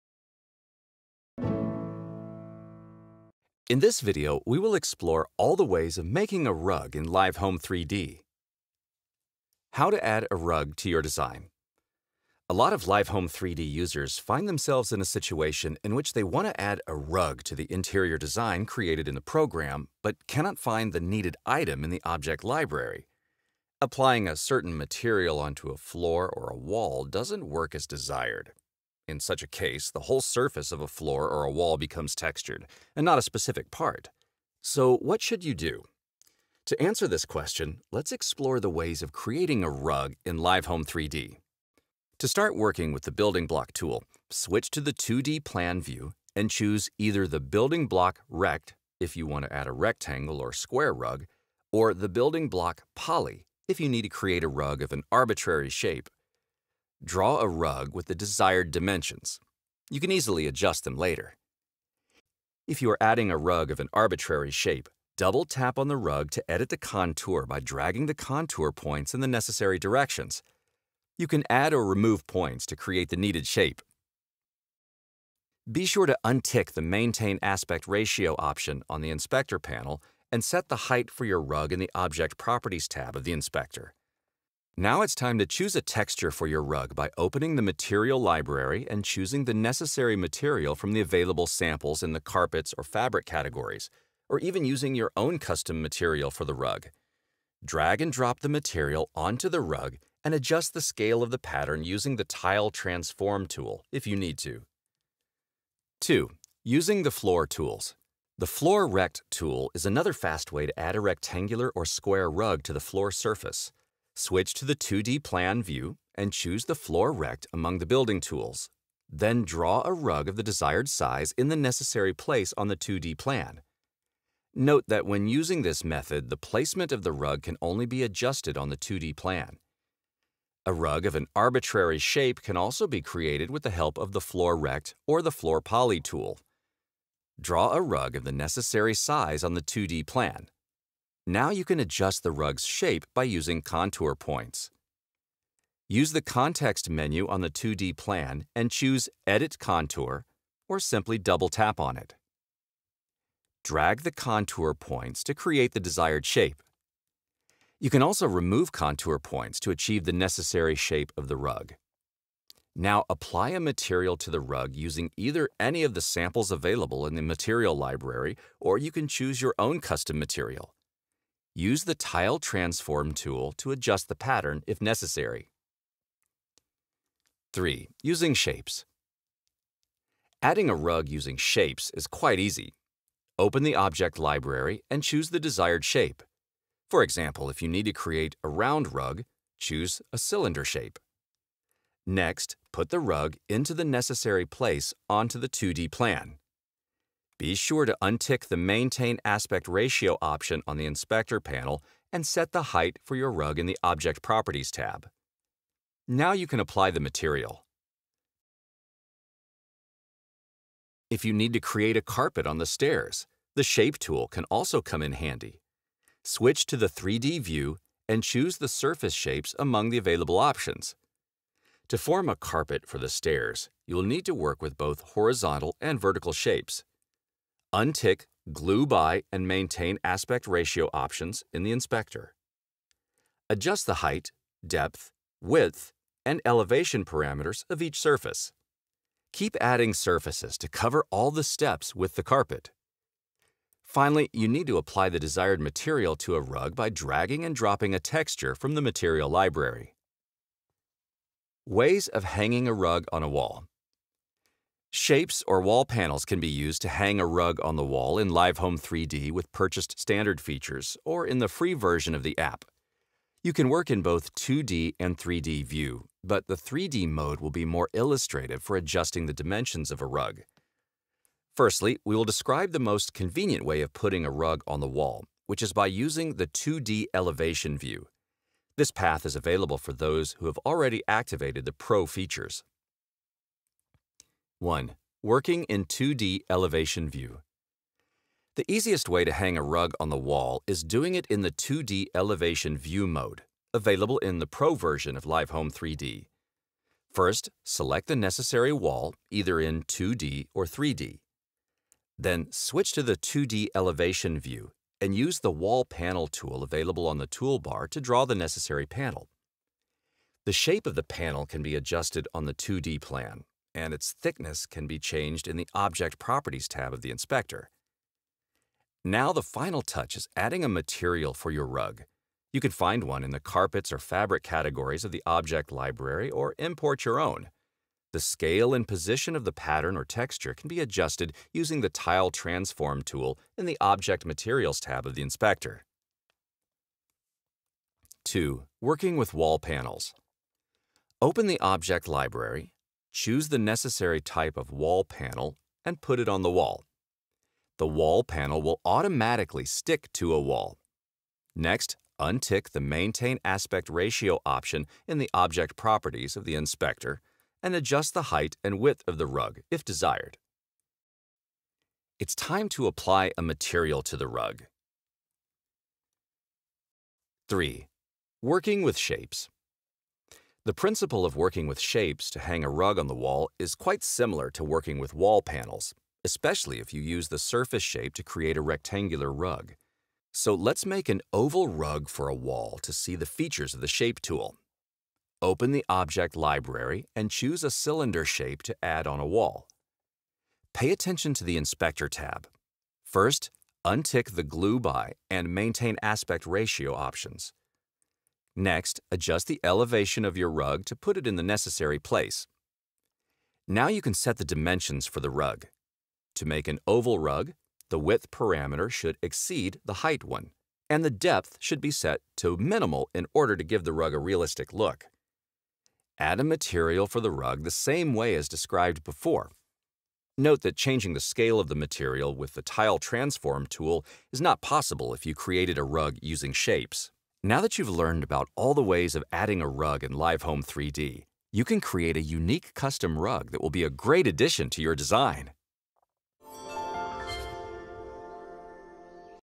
Creating a Rug - Live Home 3D for iOS/iPadOS Tutorials
In this video we will explore all the ways of making a rug in Live Home 3D for iOS/iPadOS platform.

Live Home 3D on iPads and iPhones offers several tools and objects that can help you create a rug and apply the necessary material to it afterwards. The way that should be chosen depends on whether you need to create a rug on the floor or on the wall, as well as the features that are activated—Standard or Pro. But don’t worry, you can easily create a rug even in the free version of the app. Simply follow the instructions that we provide later in this video.

Ways of Adding a Rug on a Floor

Let’s start with the options available for adding a rug on a floor surface. We will begin with the easiest way of creating a rug using the Building Block tool, available in Live Home 3D with activated Pro features on your iPad or iPhone.
1. Using Building Block Tool
This is the most convenient way for making a rug, available in the Pro version of Live Home 3D for iPads and iPhones. One of the advantages of this method is that you can adjust the parameters of the Building Block on both 2D Plan and in 3D view via the Object Properties of the Inspector. Another benefit of using this tool is being able to adjust the placement of your rug in 3D view.

To start working with the Building Block tool, switch to the 2D Plan view and choose either the Building Block Rect if you want to add a rectangular or square rug, or the Building Block Poly if you need to create a rug of an arbitrary shape. Draw a rug with the desired dimensions. You can also easily adjust them later.

2. Using the Floor Tools
A rug of an arbitrary shape can also be created with the help of the Floor Rect or the Floor Poly tool. Draw a rug of the necessary size on the 2D plan. Now, you can adjust the rug’s shape by using contour points. Use the context menu on the 2D plan and choose Edit Contour (or simply double tap on it). Drag the contour points to create the desired shape. Now apply a material to the rug using either any of the samples available in the Material library, or you can choose your own custom material.

3. Using Shapes
Adding a rug using Shapes is quite easy. Open the Object library and choose the desired shape. For example, if you need to create a round rug, choose a cylinder shape. Next, put the rug into the necessary place onto the 2D plan. Be sure to untick the Maintain Aspect Ratio option on the Inspector panel and set the height for your rug in the Object Properties tab. Now you can apply the material.

Ways of Hanging a Rug on a Wall

Shapes or Wall Panels can be used to hang a rug on the wall in Live Home 3D for iPads and iPhones with purchased Standard features or in the free version of the app. You can work in both 2D and 3D view, but the 3D mode will be more illustrative for adjusting the dimensions of a rug.
1. Working in 2D Elevation View
The easiest way to hang a rug on the wall is doing it in the 2D Elevation view mode, available in the Pro version of Live Home 3D for iOS/iPadOS. First, select the necessary wall either in 2D or 3D.

2. Working With Wall Panels
Open the Object library, choose the necessary type of Wall Panel and put it on the wall. The Wall Panel will automatically stick to a wall. Next, untick the Maintain Aspect Ratio option in the Object Properties of the Inspector and adjust the height and width of the rug, if desired.

3. Working With Shapes
The principle of working with Shapes to hang a rug on the wall is quite similar to working with Wall Panels, especially if you use the surface shape to create a rectangular rug. So, let’s make an oval rug for a wall to see the features of the Shape tool. Open the Object Library and choose a cylinder shape to add on a wall.

Now that you have learned about all the ways of adding a rug in Live Home 3D for iPads and iPhones, you can create a unique custom rug that will be a great addition to your design!

Timecodes:
0:00 Intro
0:42 Adding a rug on the floor using Building Block tools
1:46 Adding a rug on the floor using the Floor tools
2:57 Adding a rug on the floor using Shapes
3:30 Creating a carpet on the stairs
4:11 Ways of hanging a rug on a wall
4:44 Hanging a rug on a wall working in 2D Elevation View
5:37 Hanging a rug on a wall working with wall panels
6:07 Hanging a rug on a wall working with Shapes